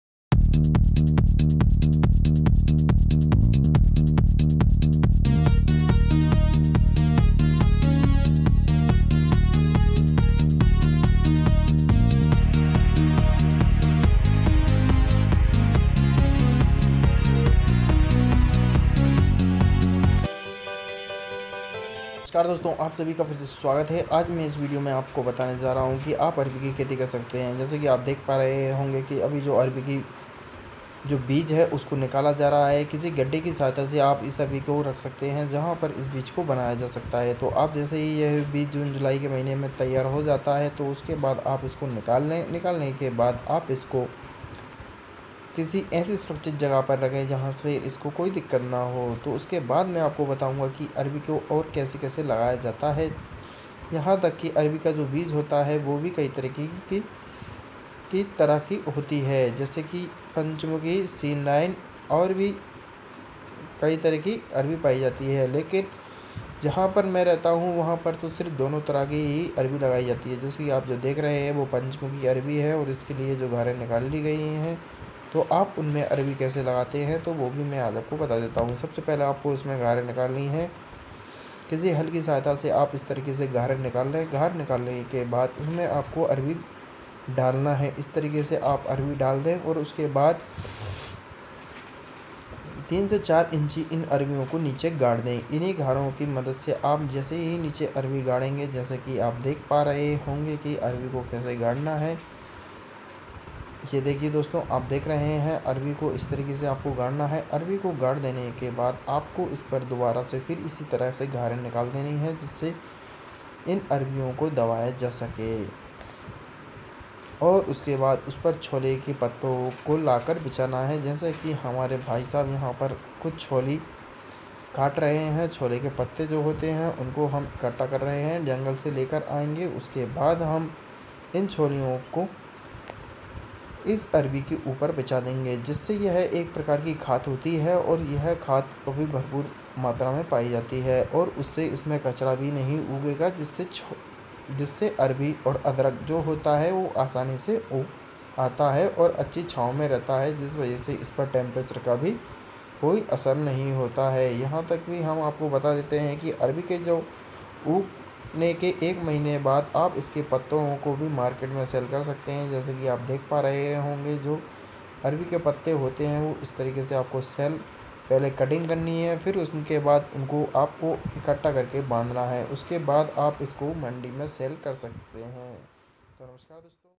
How to cultivate Arabic! अरबी की खेती कैसे करते है पूरी जानकारी
दोस्तों इस वीडियो में हमने आपको बताया है कि अरबी की खेती कैसे करते हैं और इसको कैसे करना चाहिए हेलो और साथ में यह भी बताया है कि आप इसको कैसे लगाएंगे आप इसके ऊपर कैसे छोलिया बिछाएंगे तो अगर वीडियो आपको अच्छा लगे तो धधन्यवाद